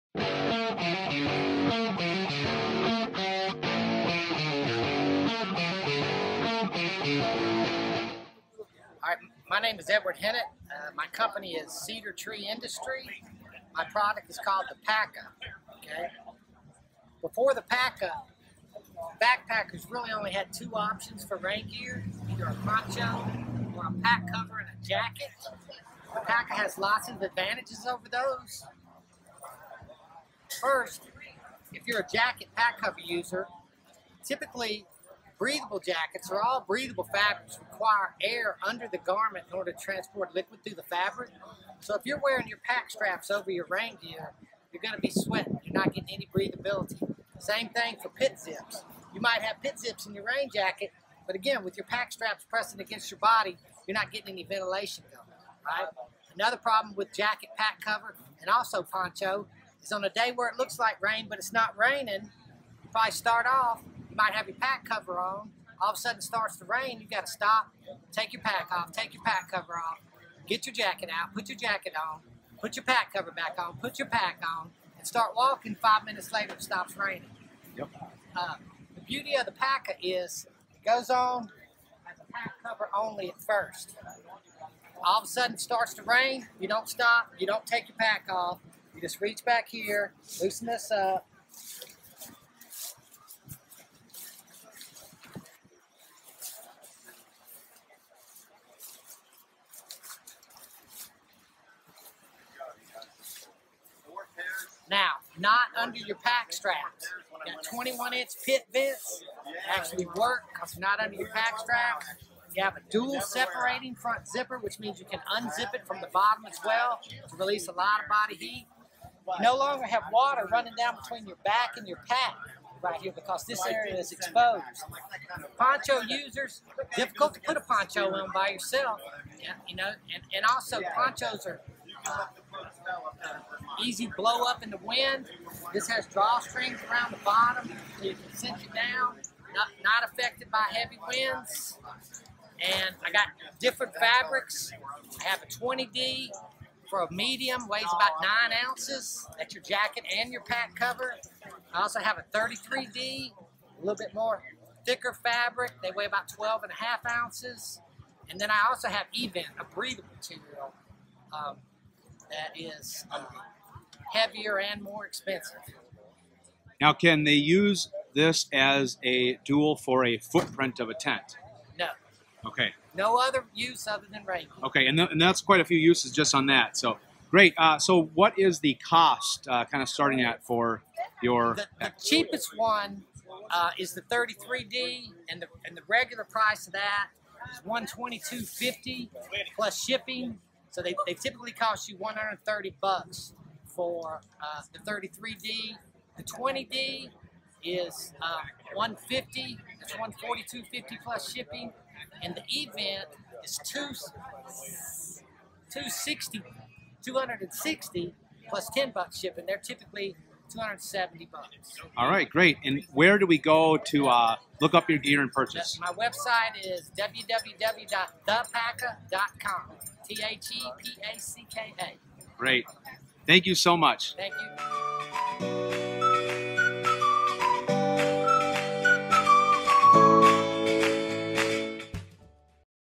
Trail Days Gear Vendors: The Packa
During our weekend in Damascus, I caught up with some of the biggest names in the backpacking world to give us a peek at their latest and greatest gear. This video covers a new intuitive piece of gear called The Packa.